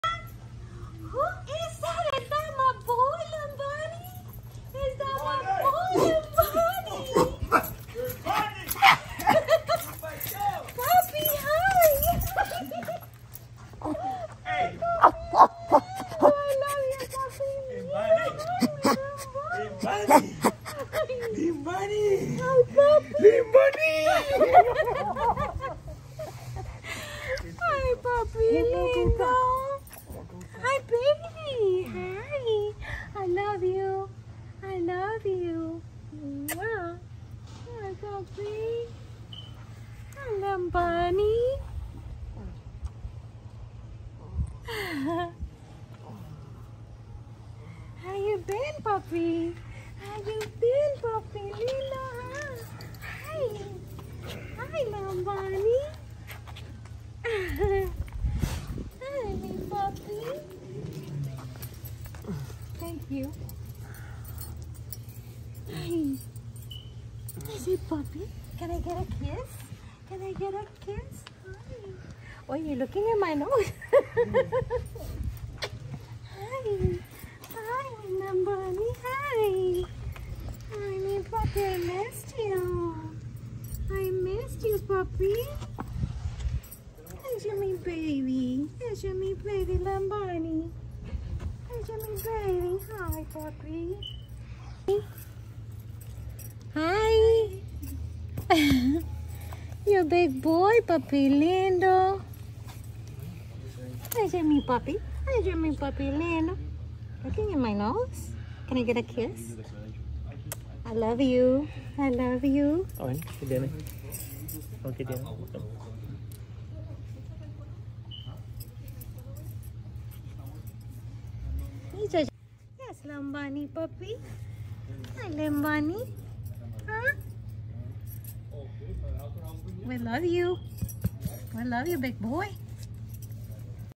When we go see CHIMP LIMBANI from @zwfmiami Our forever big baby! ❤️❤️❤️❤️❤️❤️
You can see Limbani’s story in our video “A Little About Our Story” but long story short…Limbani got to Zoological Wildlife Foundation when he was just a few days old with 7 broken ribs and pneumonia, at the time there was construction going on and we took him in, like we’ve taken many other babies, to help take care of him to give him his medication, attention and love. Going back and forth he stayed for about 7 months and he will never forget and will forever be grateful. Limbani is 5 yrs old and this is how he treats us every  time we go see him…he just fills our hearts…with his hugs, his eyes, his smile, his love ❤️ ❤️❤️❤️❤️❤️❤️❤️❤️ HOPE WE FILL YOURS WITH JOY WITH THIS VIDEO!